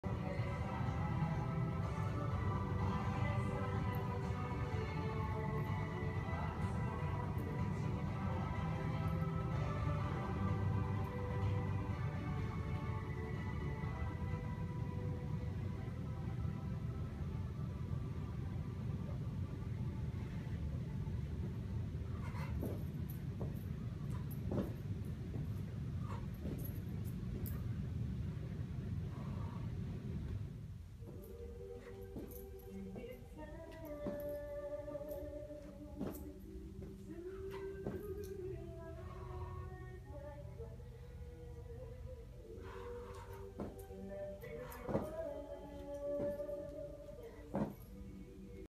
Squat with Rotation Progression | Rippel Effect Fitness
1. Squat with Rotational Step
2. Drop Squat with Rotation (kinda goofed up so I did a few reps of these twice) :)
3. Squat Jump with Rotation